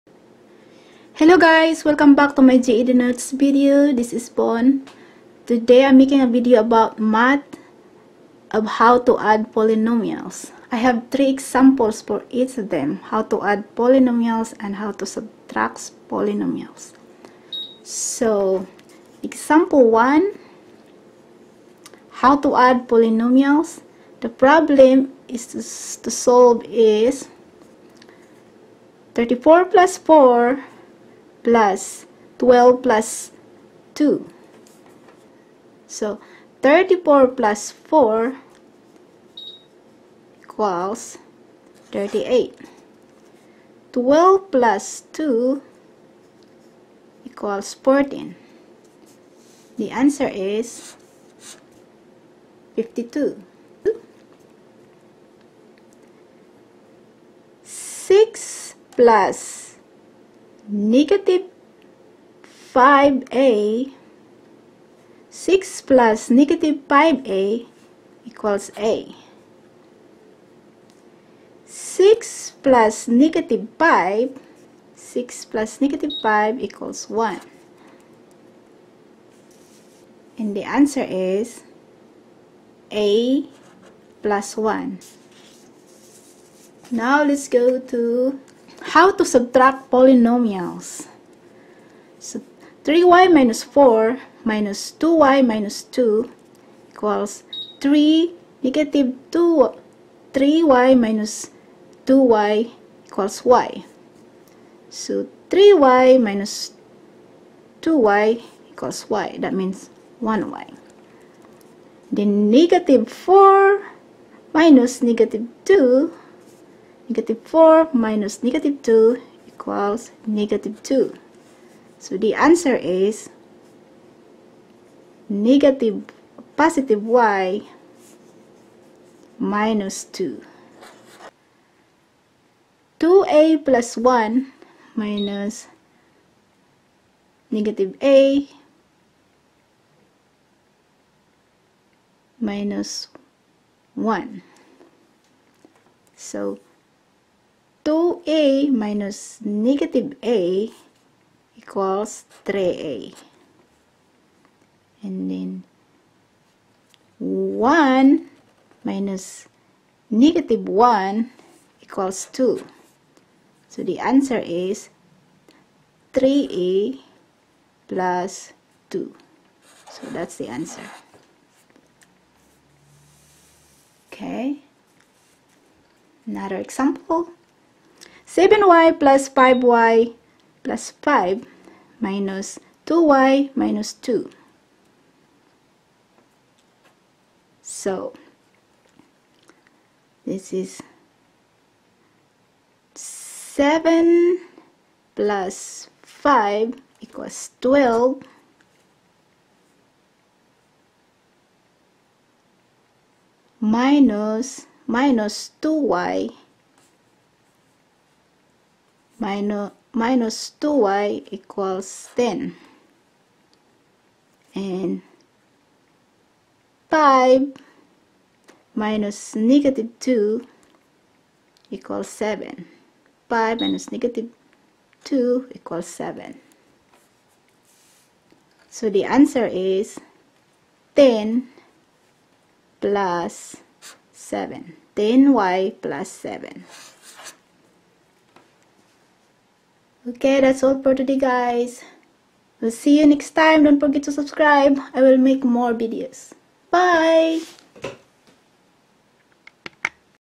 GED math,how to Add and subtract polynomials.
GED math 5 examples for how to add and subtract polynomials.